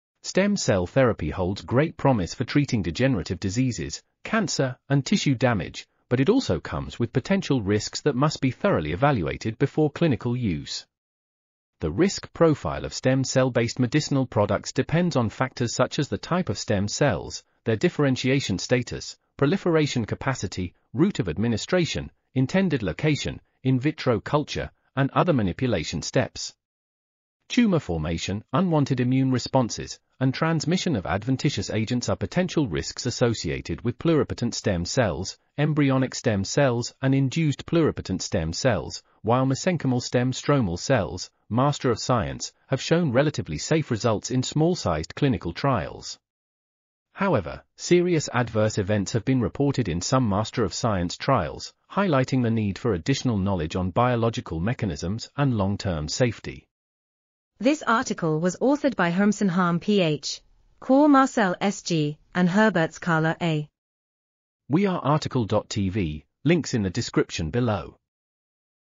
Risk factors in the development of stem cell therapy | RTCL.TV
### Keywords ###

### Article Attribution ###
Title: Risk factors in the development of stem cell therapy
Authors: Hermsen Harm PH, Kwa Marcel SG ,and Herberts Carla A
Publisher: BMC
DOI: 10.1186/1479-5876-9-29

### Video Timestamps ###
0:00:00 - Summary
0:00:57 - Title
0:01:04 - Tail
0:01:08 - End